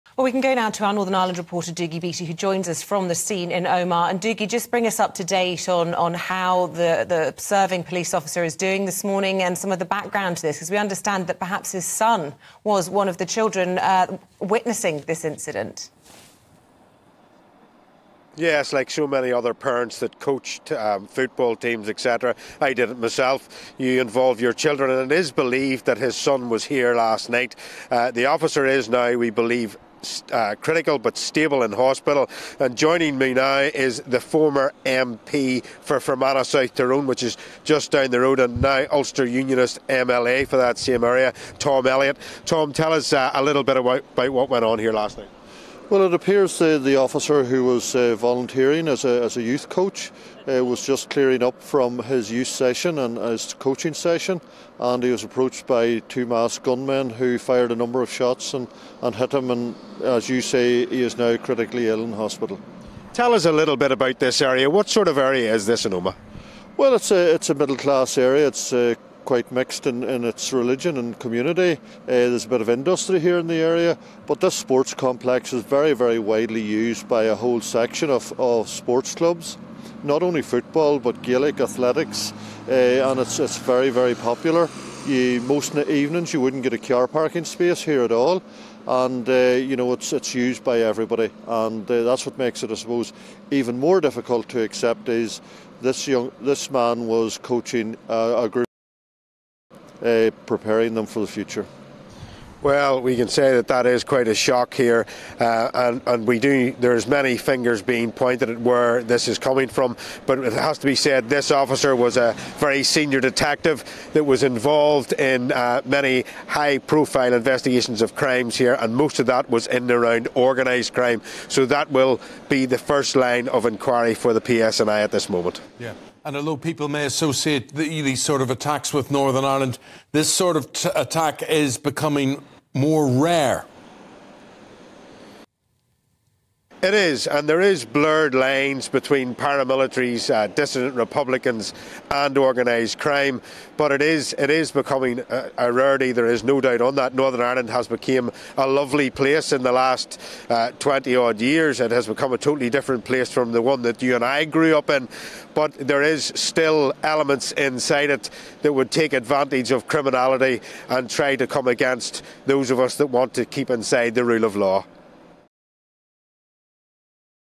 'He was preparing young footballers for the future' UUP MLA Tom Elliot on the Omagh shooting
'This man was coaching a group of young footballers, preparing them for the future.'

UUP MLA, Tom Elliot, speaks to GB News' Dougie Beattie about the shooting of a PSNI officer in Omagh while he was coaching a Gaelic football match.